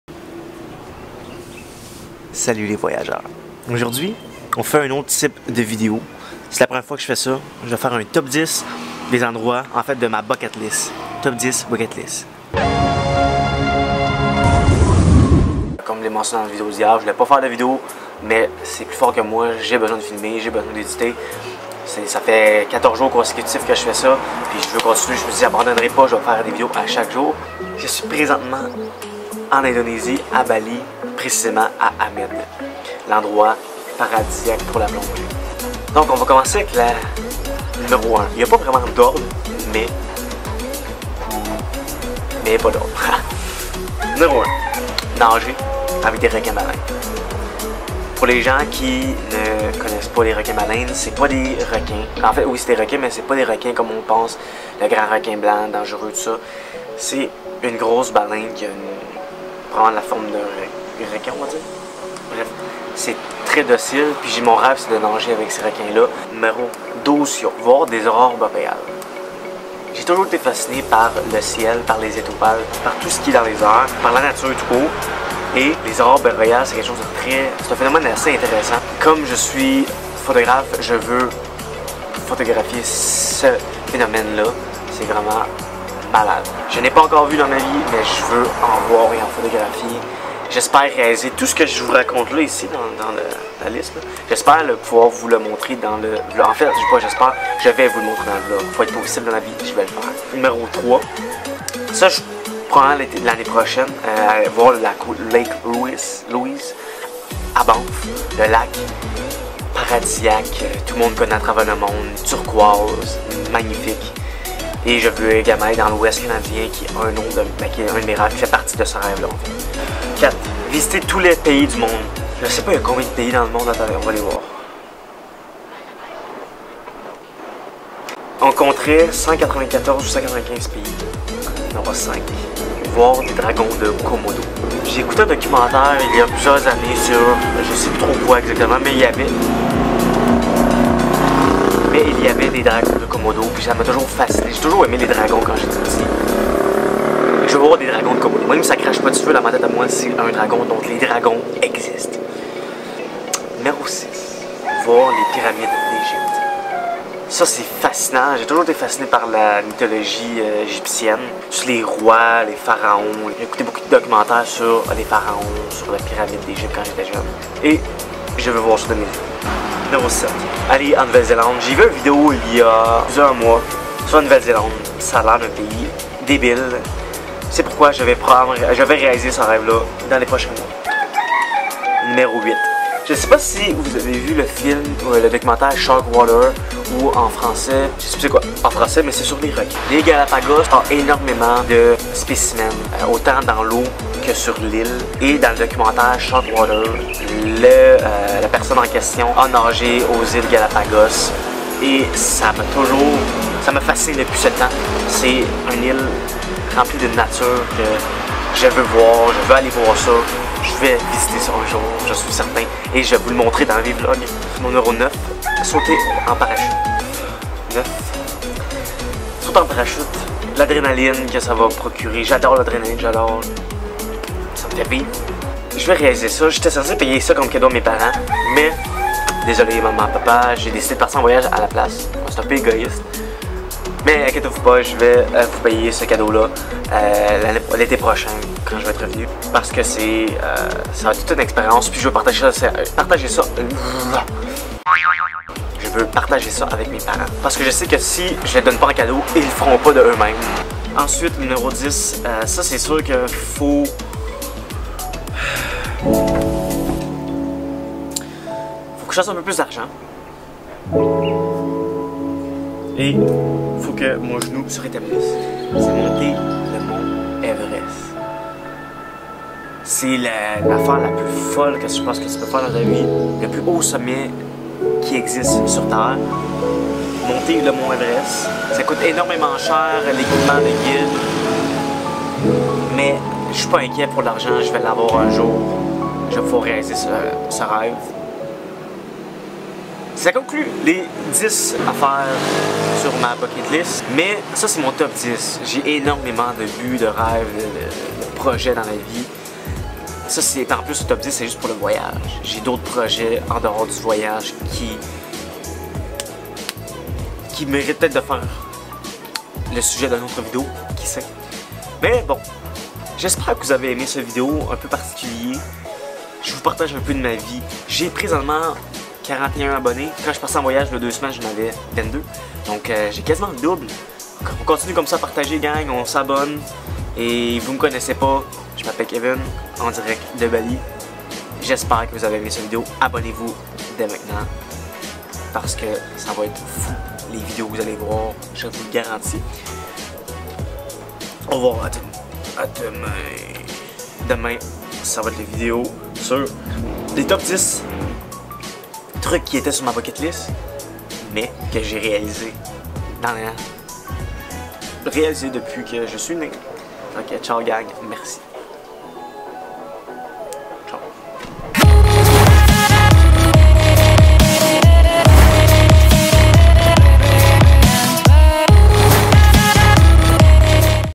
Le top 10 des trucs sur ma Bucketlist de voyage ~ Vlog 30
Nous avons tous des rêves, une liste de choses que l'on veut accomplir avant notre mort. Voici la mienne!

• Holly Drummond!

Facebook: Vue De Mes Yeux
Instagram: Vue de mes yeux